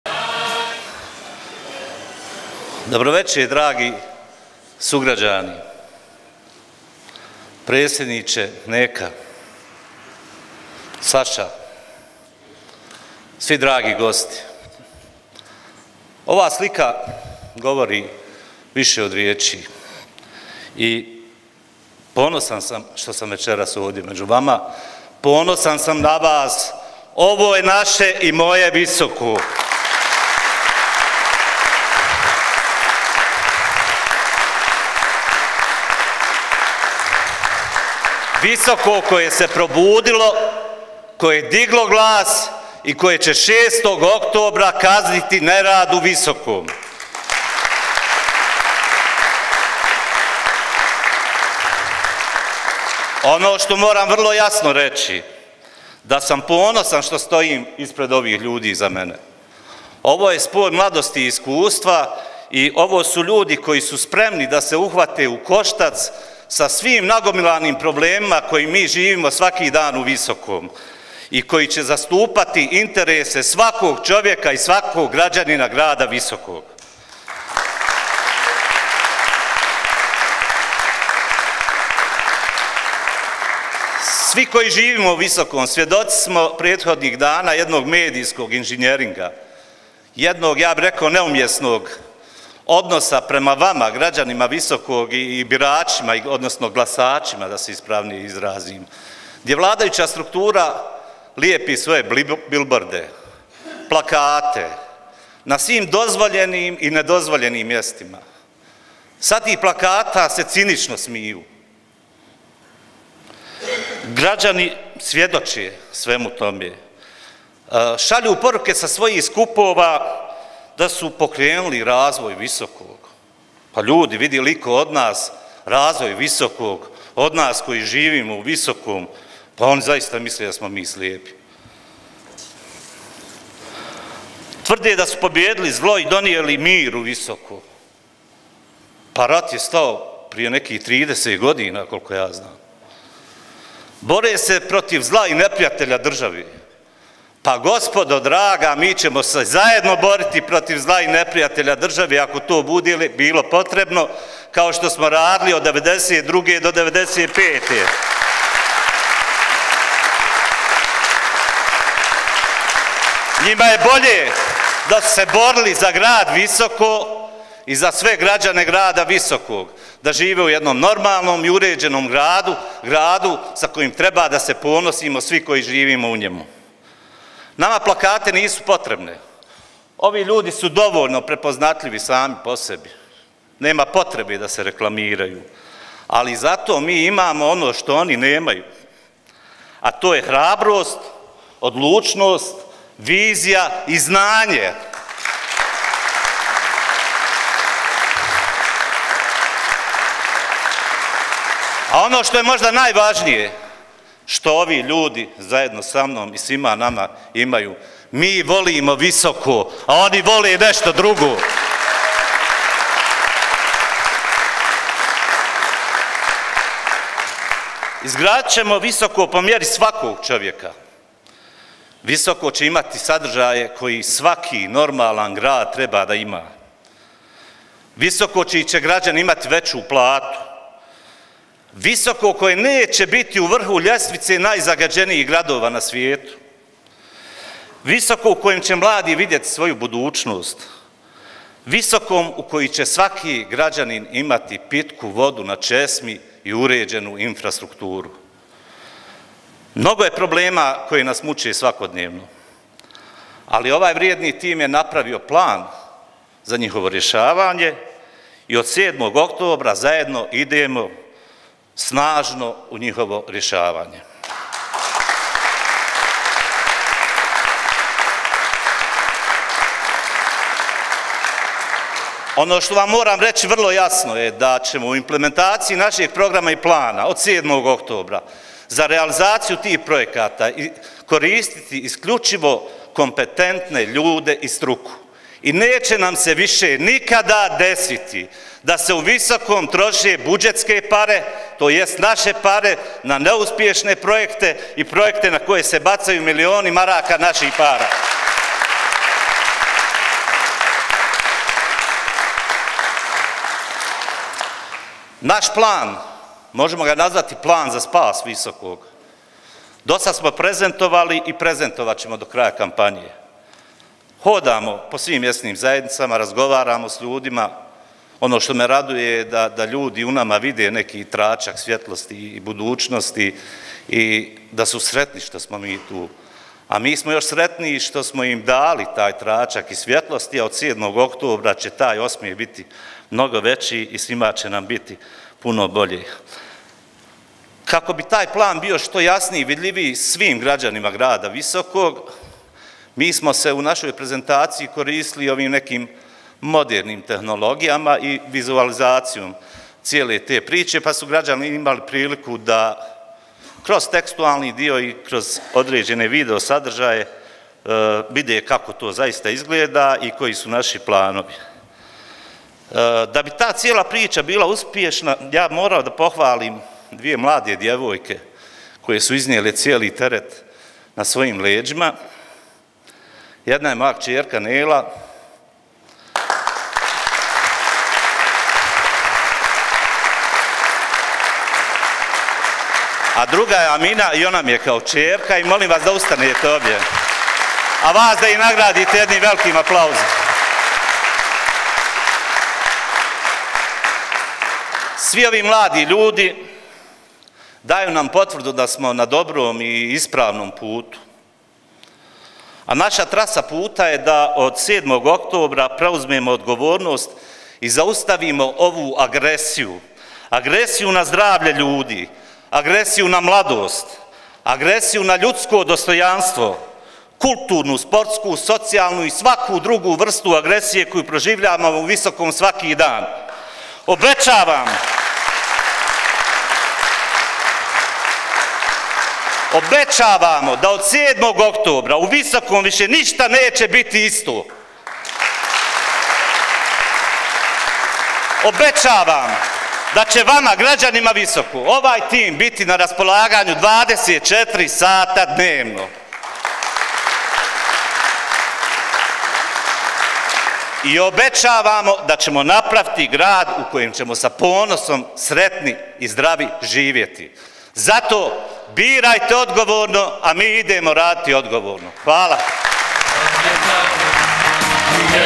Delić: Moramo zaustaviti ovu agresiju na zdravlje, mladost, dostojanstvo
Adnan Delić